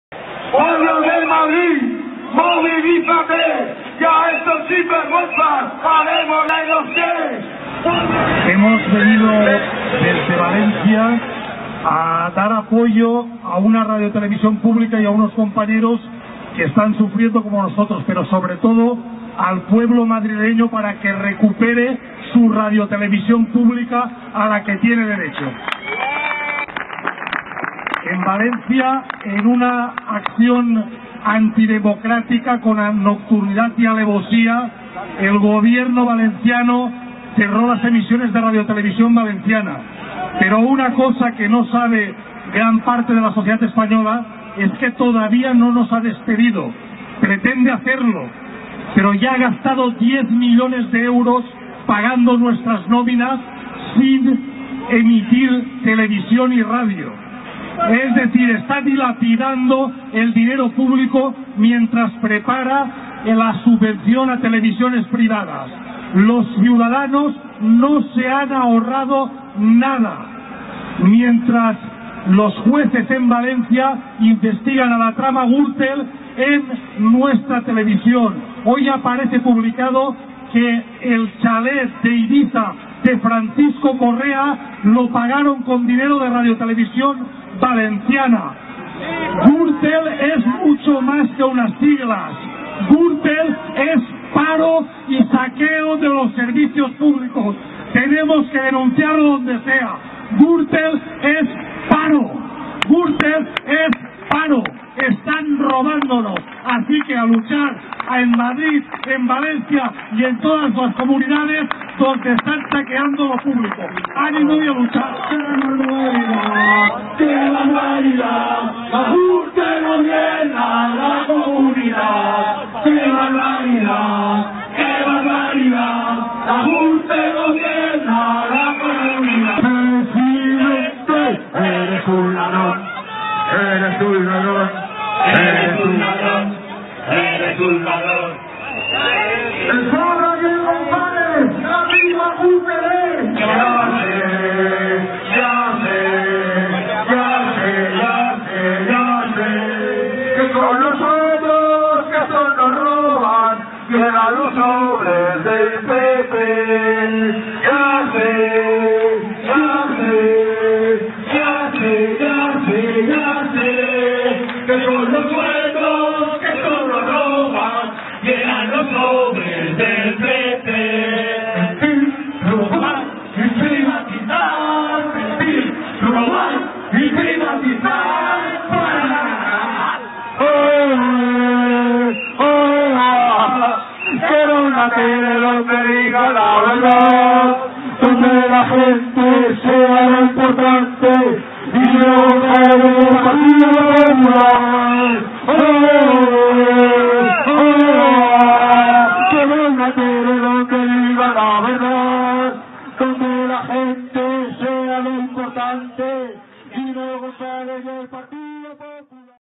Manifestació Telemadrid 12 01 2014
Treballadors de RTVV en suport als treballadors de Telemadrid. 12 gener 2014.
Vicent Mifsud: "Gurtel és Atur". Semblances de la Comunitat de Madrid i la Comunitat Valenciana: les dues tenen Presidents no elegits a en les urnes, les dues comparteixen la trama corrupta Gürtel, les dues tenen les seves radiotelevisions públiques amenaçades, a les dues les governa el Partit Popular.